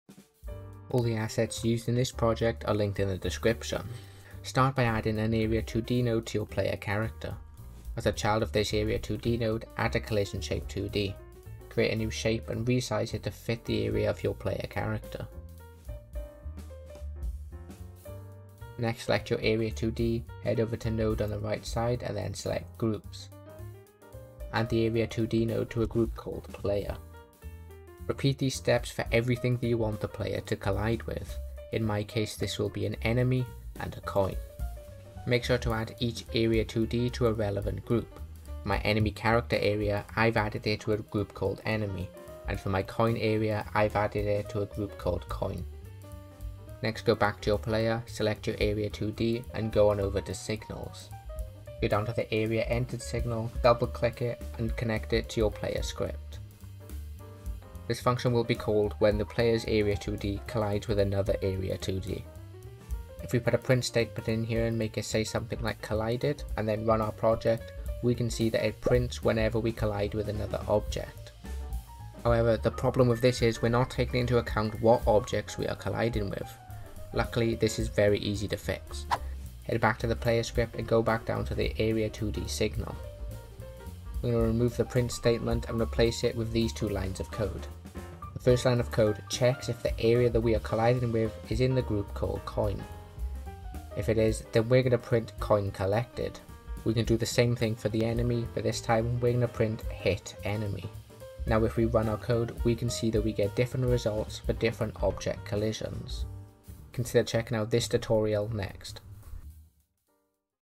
How To Detect Collisions Using Groups In Godot
Here's how you can detect collisions in the Godot Engine using Area2D's and groups.